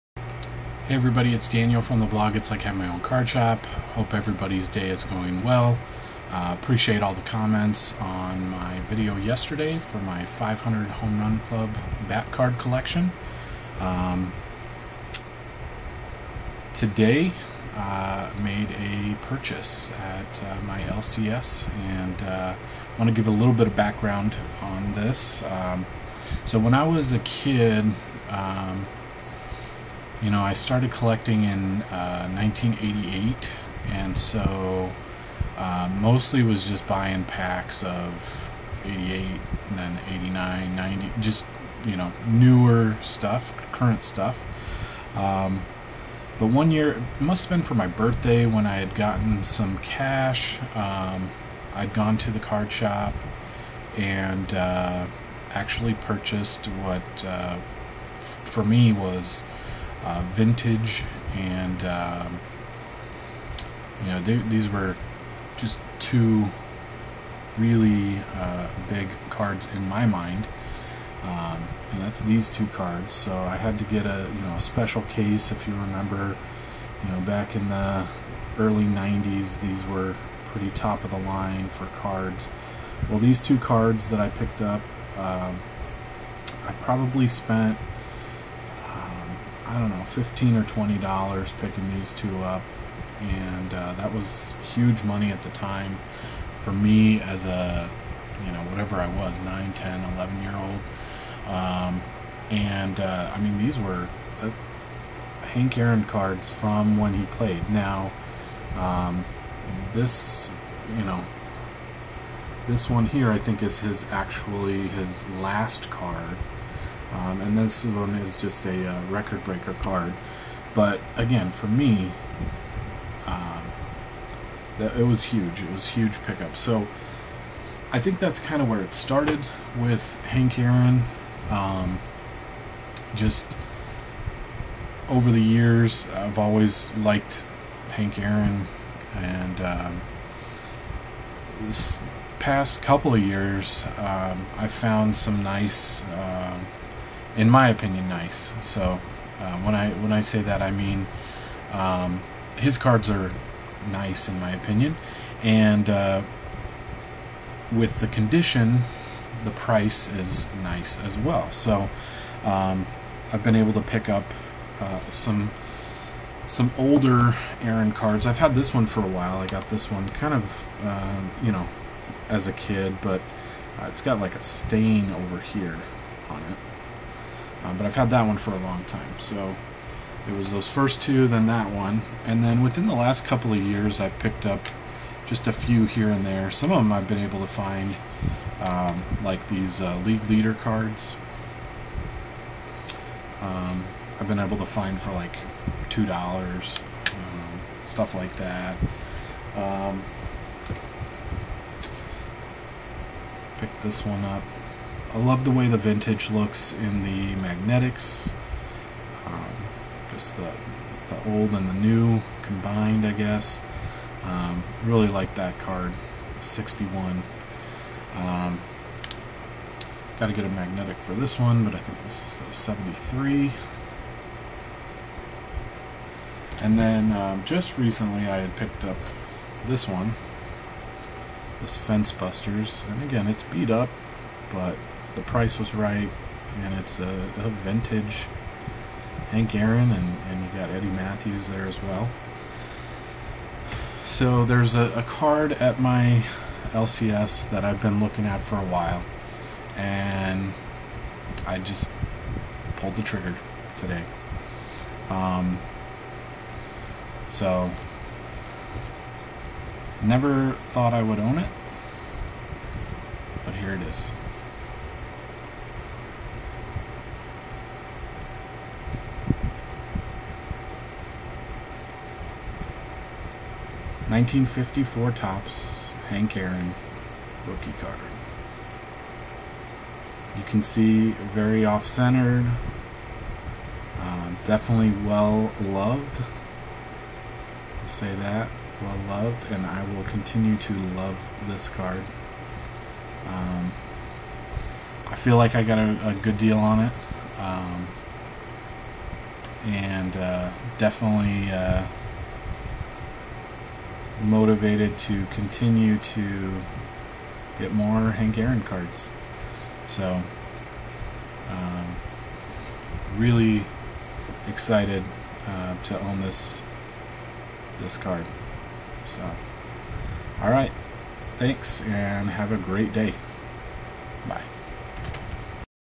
Never thought I would EVER own this card!
I pulled the trigger today on a 1954 Topps Henry Aaron RC!